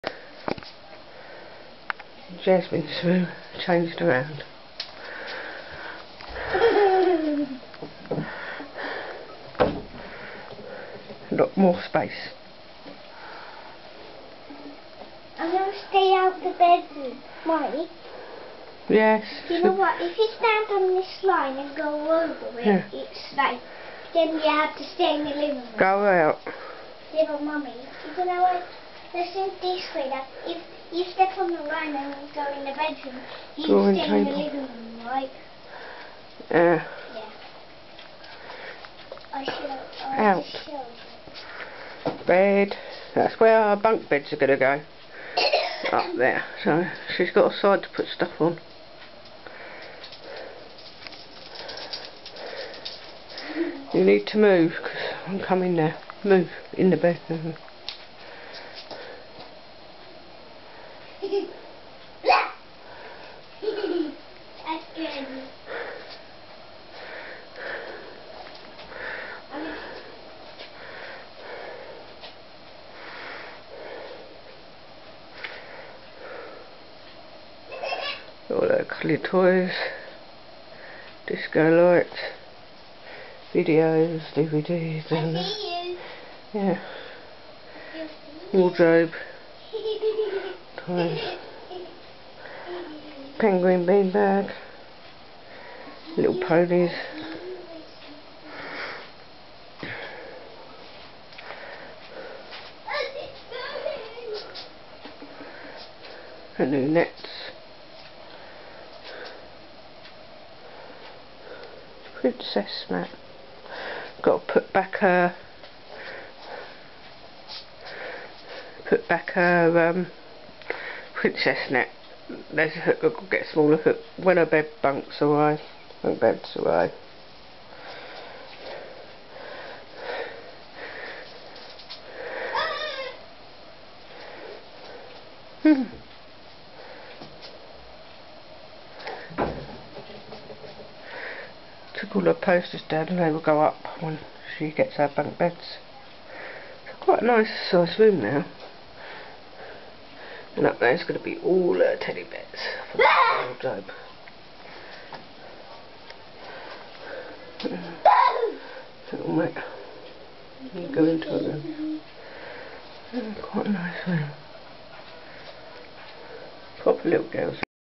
Jasmins bedroom change around
after I cleaned her room and moved thing around ready for her new bunk beds. Now she has more space.